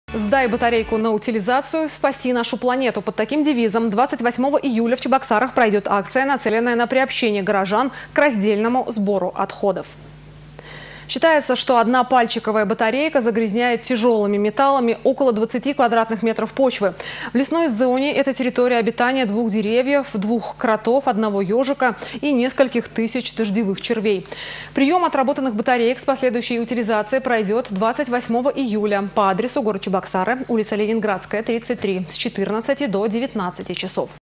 Сдай батарейку на утилизацию – спаси нашу планету
«Сдай батарейку на утилизацию – спаси нашу планету» под таким девизом 28 июля в Чебоксарах пройдет акция, нацеленная на приобщение горожан к раздельному сбору отходов.